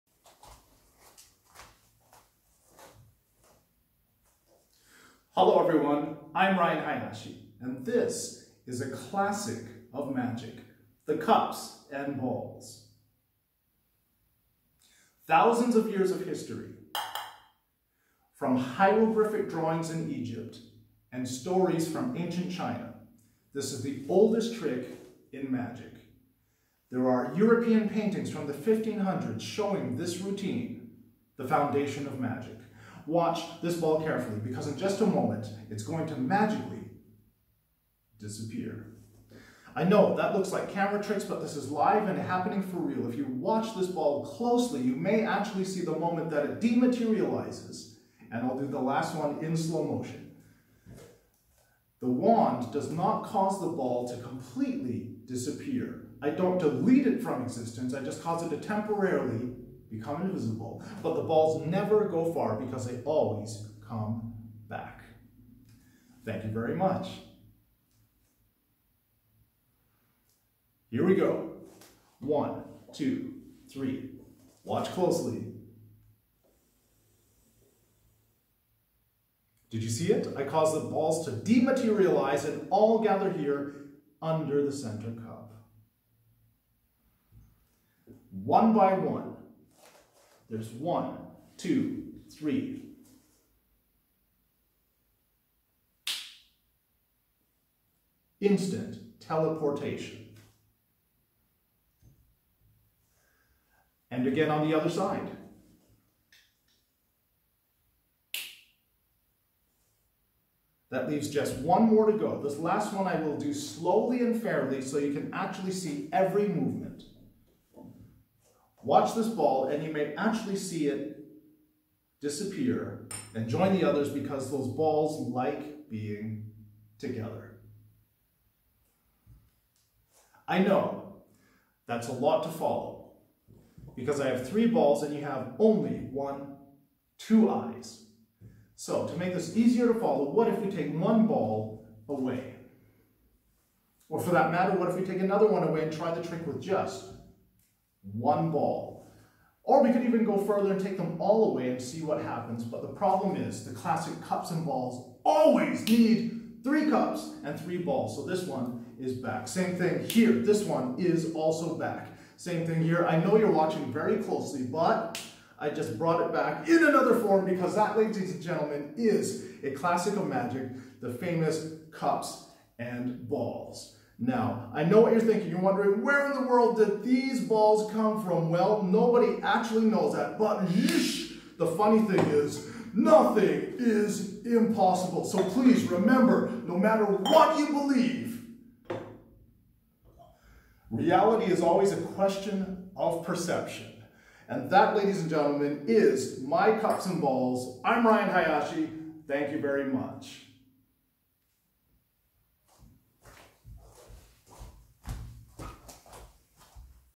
RYAN HAYASHI - Episode 20: The Psychological Damage Magic Series - Ultimate Cups and Balls
Ryan Hayashi, the Asian Alpha of Magic, performs his Ultimate Cups and Balls. This was recorded at Hayashi Dojo, his studio in Mannheim, Germany, where he teaches magic and the martial arts.